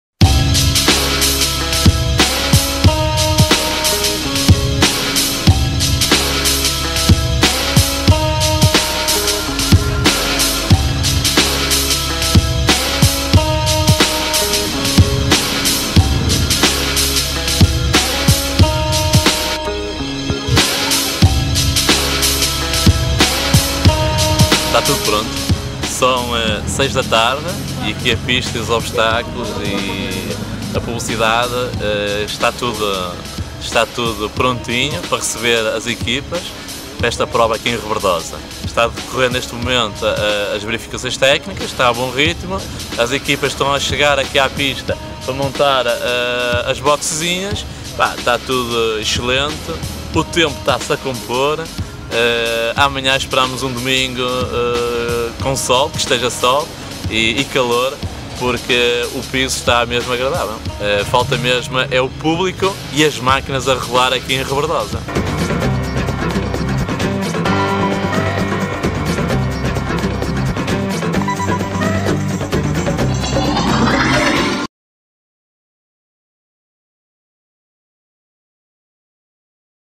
Domingo Fantástico de Trial 4x4 - Rebordosa
Está tudo pronto para as decisões que encerram o Campeonato Nacional de Trial 4x4.